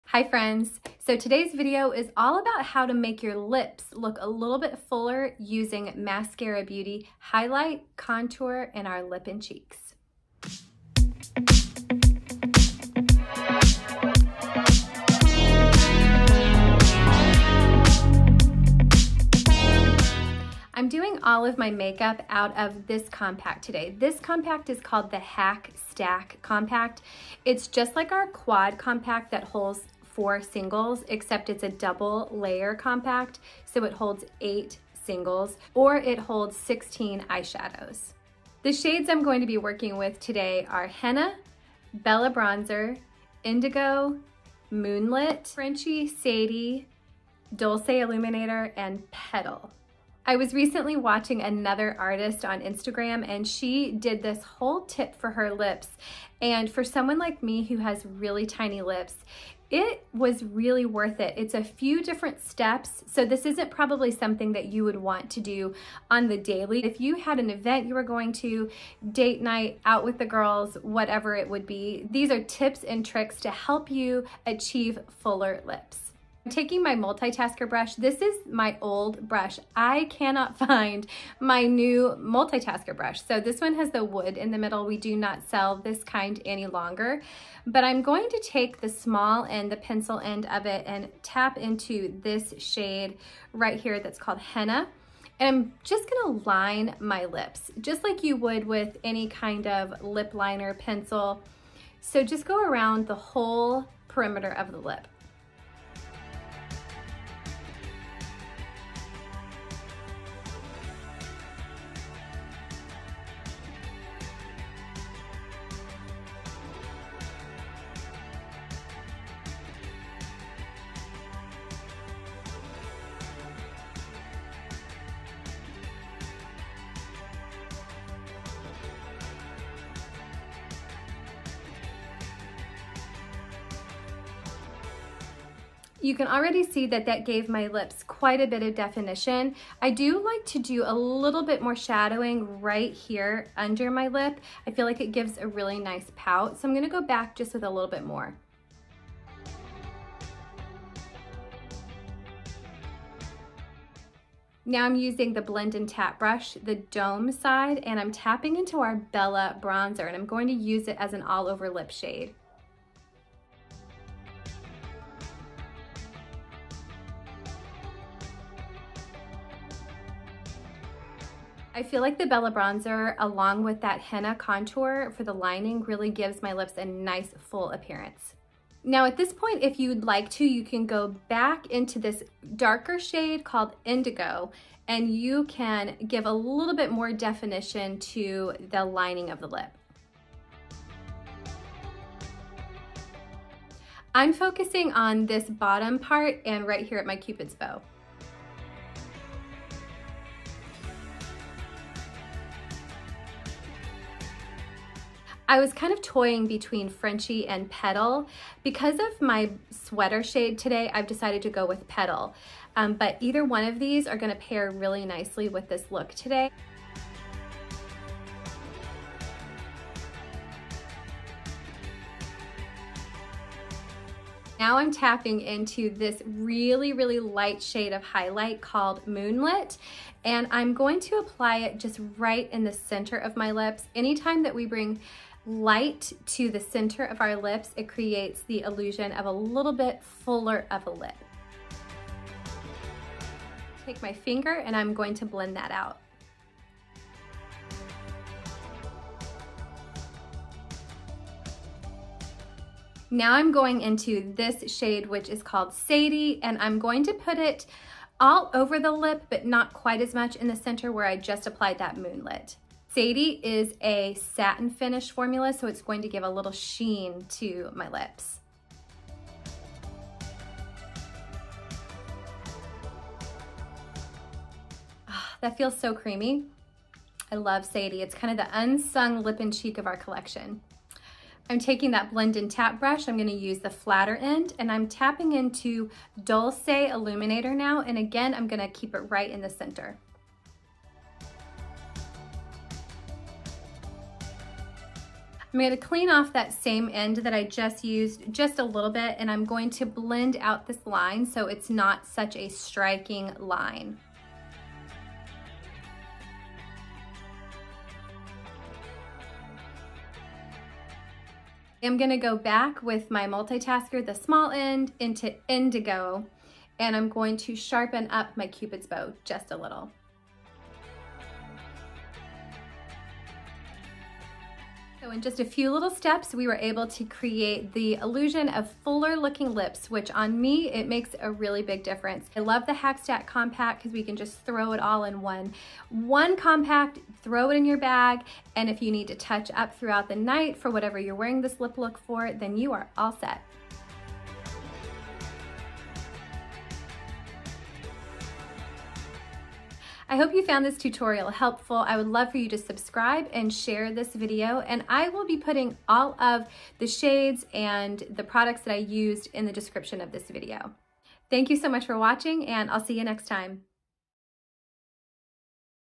How To Achieve Fuller Lips with SEINT (Formerly Maskcara Beauty)
Hi ladies! Beth here!

SHADES USED:
* Henna Contour
* Indigo Contour
* Bella Bronzer
* Petal Lip+Cheek
* Moonlit Highlight
* Dulce Illuminator